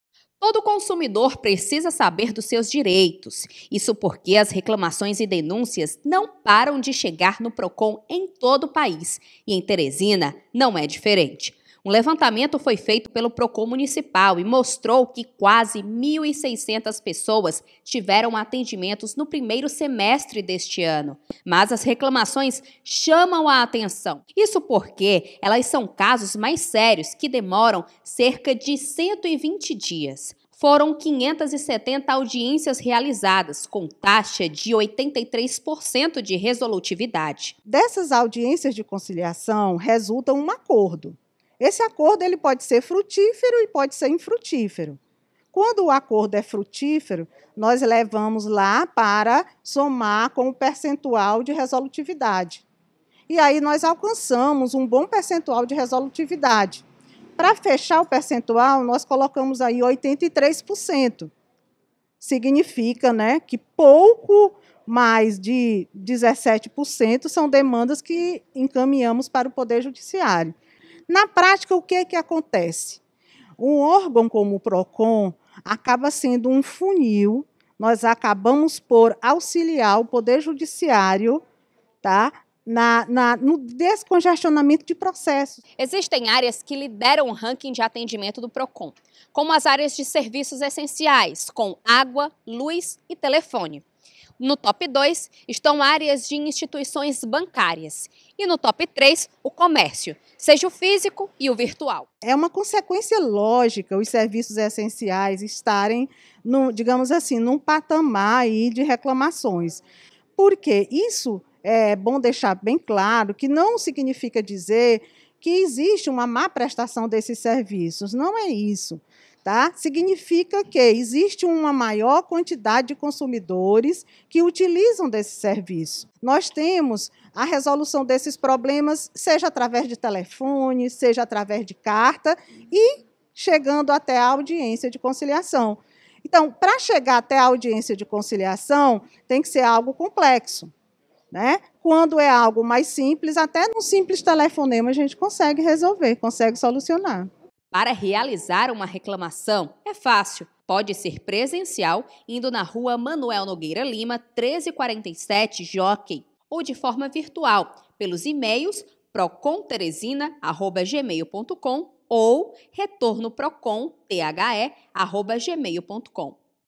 Serviços essenciais lideram reclamações no Procon Municipal
Os serviços essenciais, instituições bancarias e também o comércio estão entre os itens mais reclamados pela população teresinense. Os dados foram divulgados pelo Procon Municipal, onde mais de 1.500 pessoas buscaram pelo órgão, somente no primeiro semestre, para formalizarem reclamações, que possuem taxa superior a 80% de resolutividade. Veja mais detalhes na reportagem de Lalesca Setúbal e Roniel Matos, da TV Assembleia.